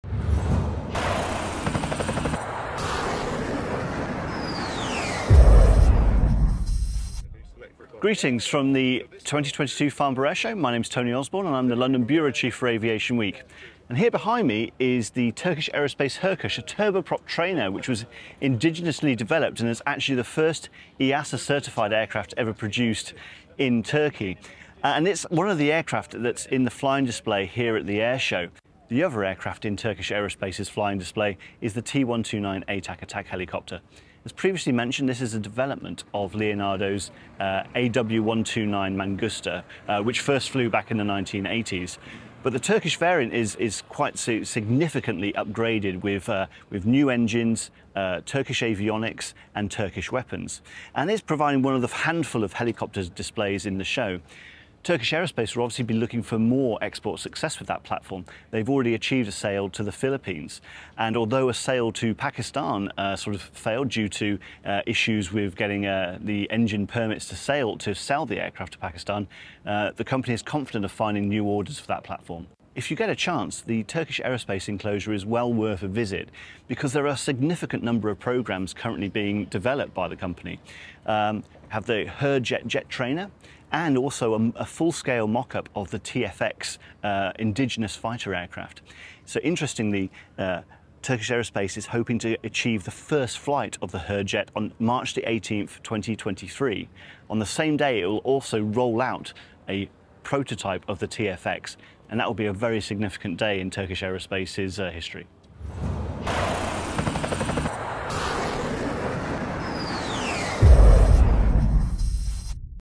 Turkish Stars in the Farnborough Flying Display
Turkish Aerospace has a major presence on the ground and in the air display at this year’s Farnborough Airshow, London Bureau chief takes look at what the Ankara-based company has bought to this year’s event.